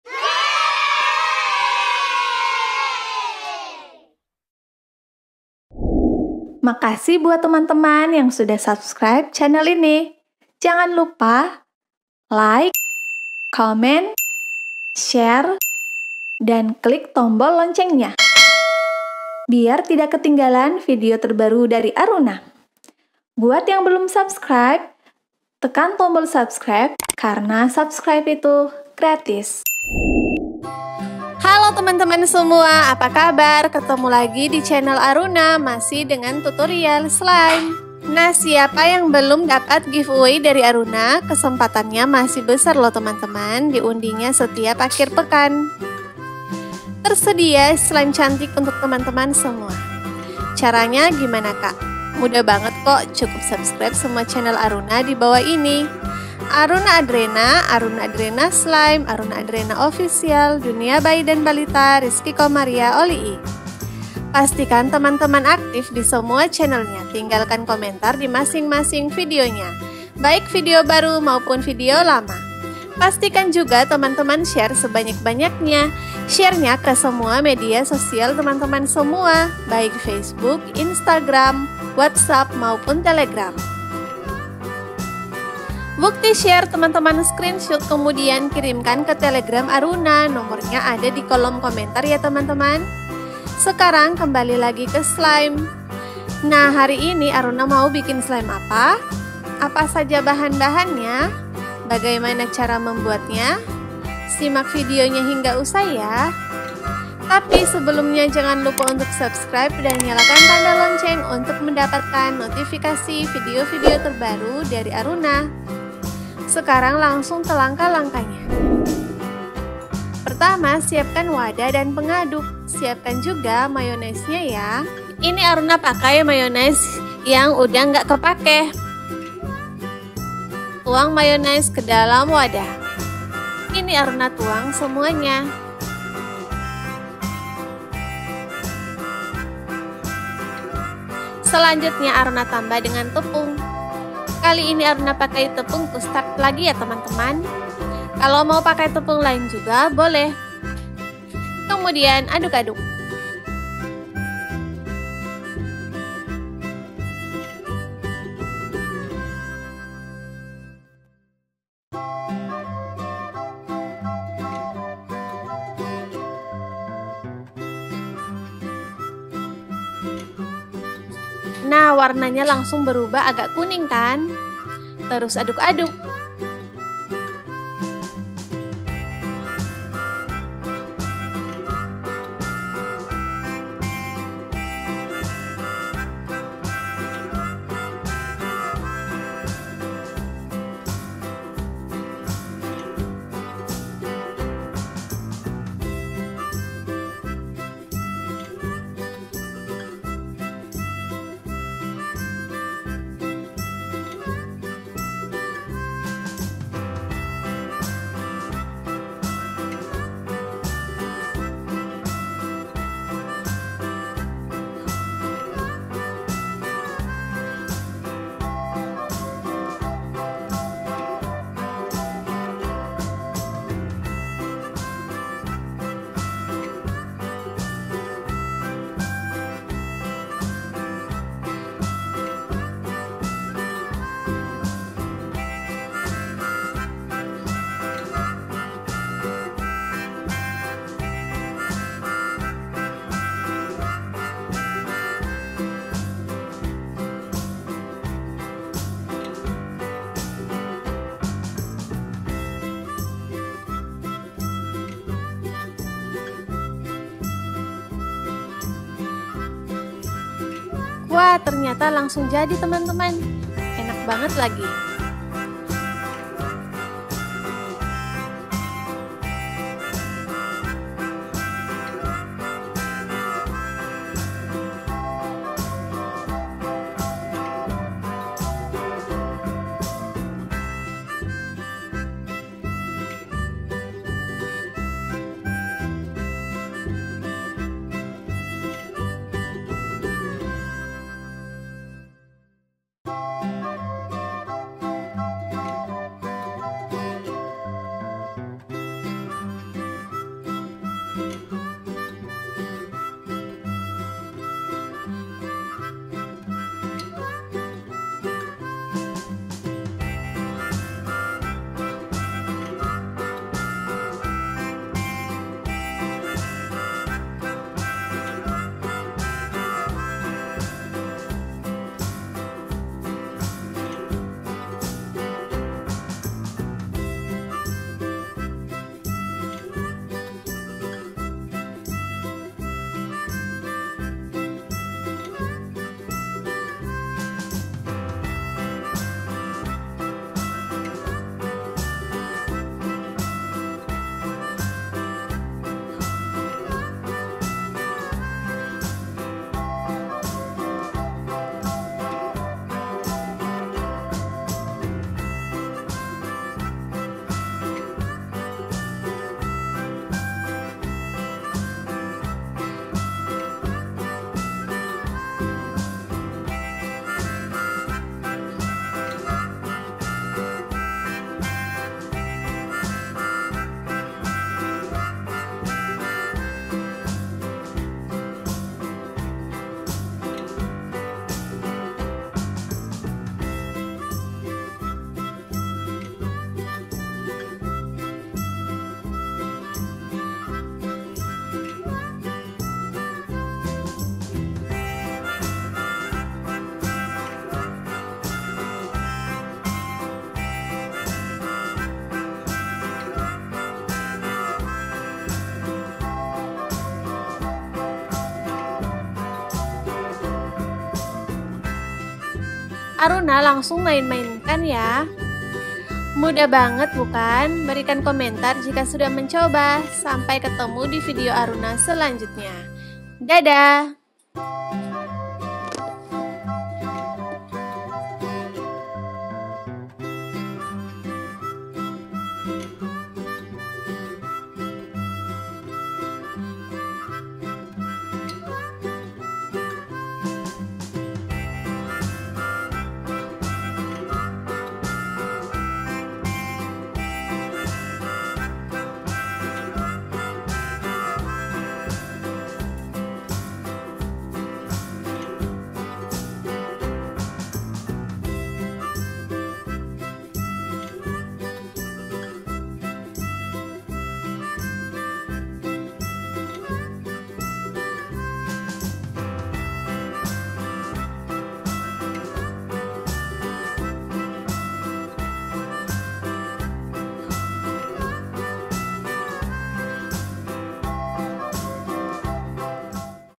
Cara Membuat Slime Dengan 2 Bahan Yang Ada Di Dapur Mudah
Hallo teman-teman semua, kali ini Aruna mau share Cara Membuat Slime Dengan 2 Bahan Yang Ada Di Dapur Mudah

Bagaimana cara membuatnya??? Simak terus videonya hingga usai yah.

Simak juga video video Aruna lain tentang, cara membuat slime, cara bikin slime, dan cara membuat slime lainnya.